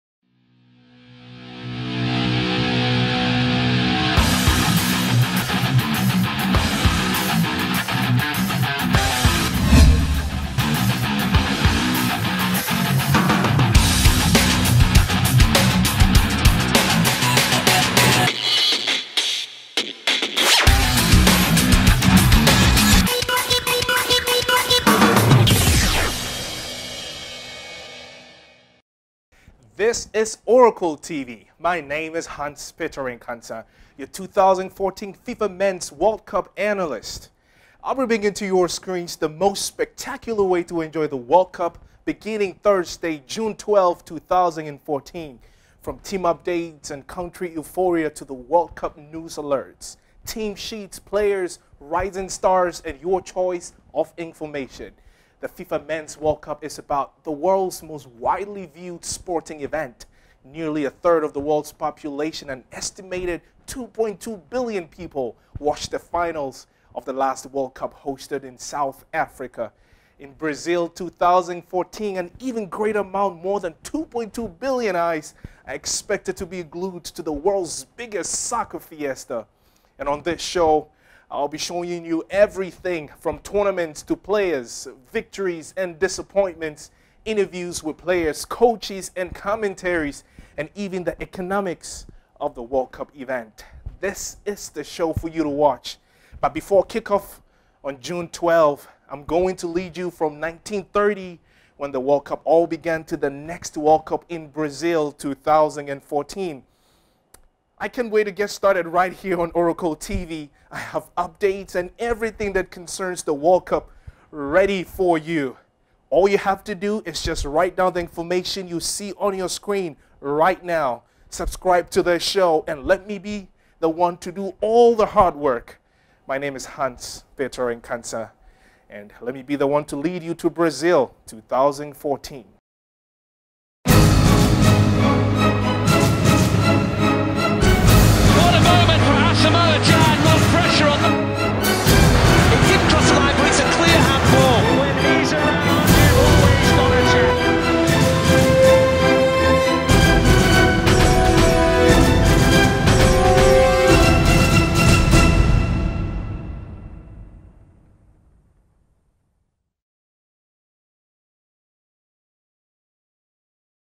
BRAZIL FIFA 2014 WORLD CUP SHOW COMING SOON!
Oracle Sports TV Presents a new telecast of the best engaging pre-FIFA Men's World Cup. Hosted by Hans-Peter Nkansah,Oracle Sports Analyst.